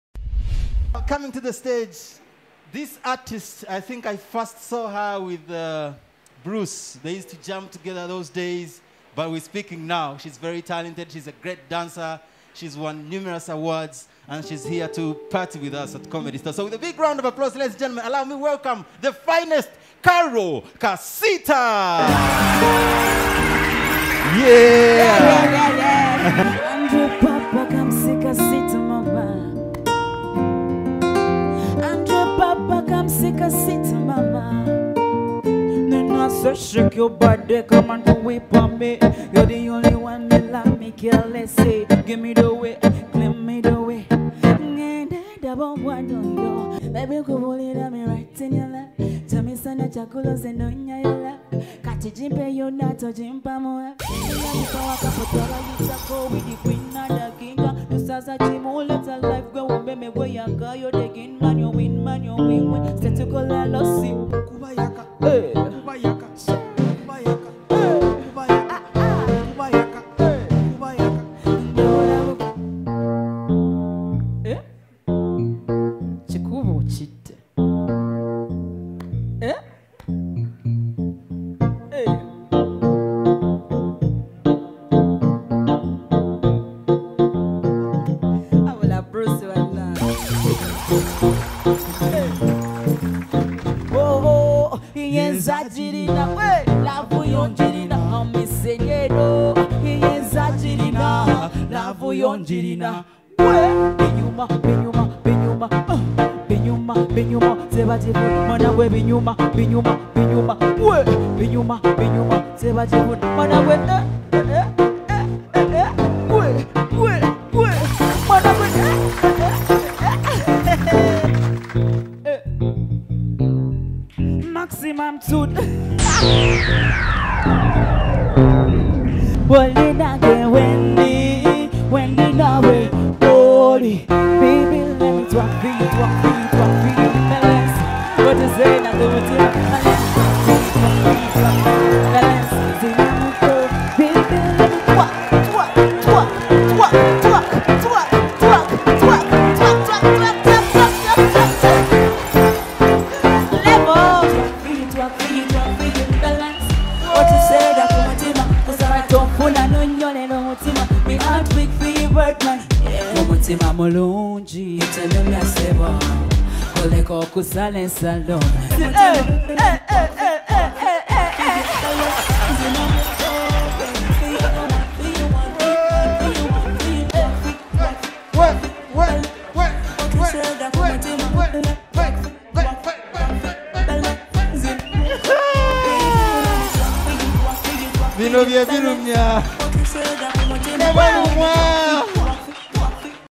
Karole Kasita Scientific Comedy Store Performance
Karole Kasita perform's at the Scientific Comedy Store in September 2020.